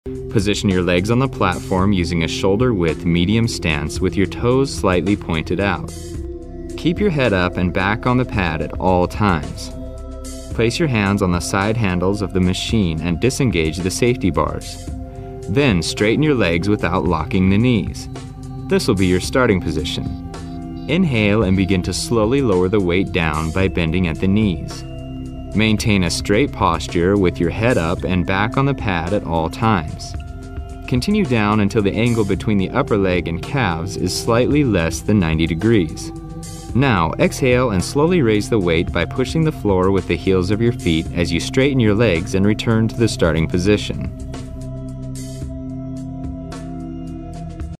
Hack Squat
Target Muscle : Legs

Perform this workout with progressive weights loading with proper form to gain maximum results.